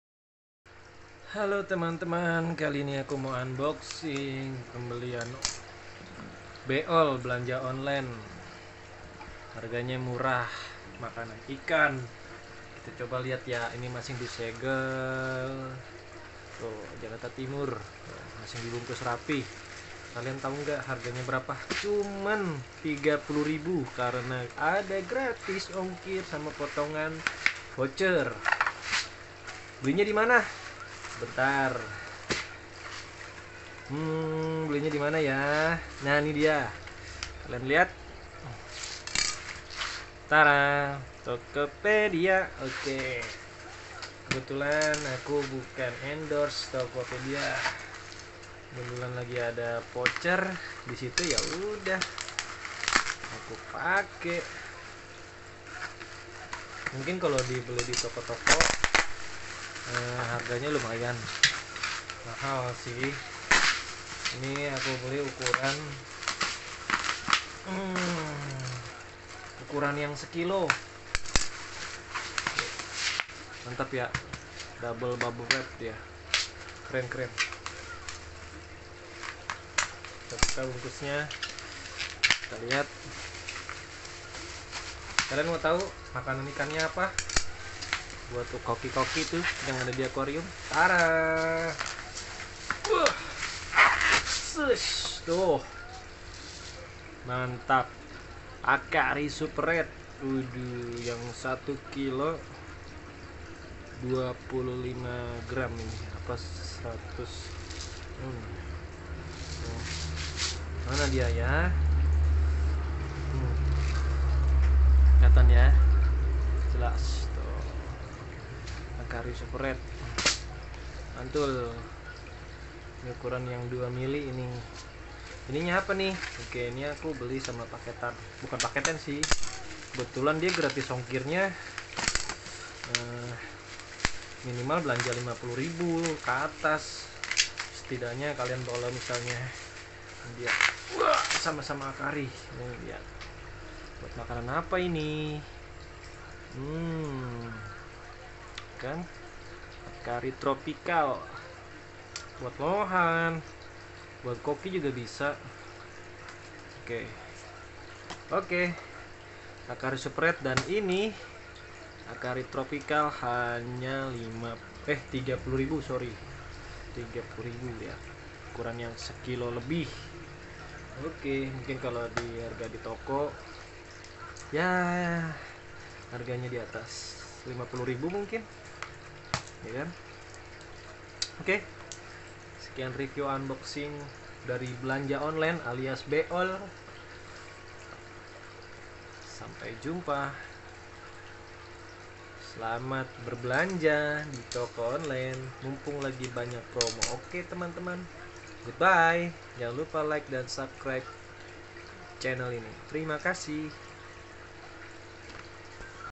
Unboxing Pakan Ikan Akari Hasil Belanja Online Dapet Harga Murah | Yt Kreasi Video
pakan ikan